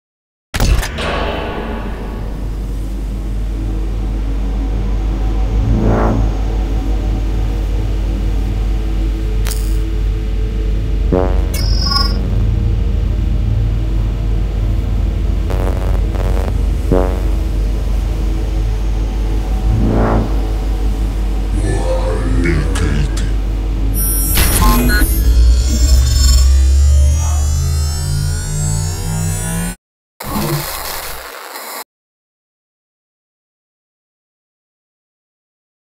BKT Teaser
Realizzazione del Teaser BKT in grafica 3D per famosa azienda di produzione pneumatici.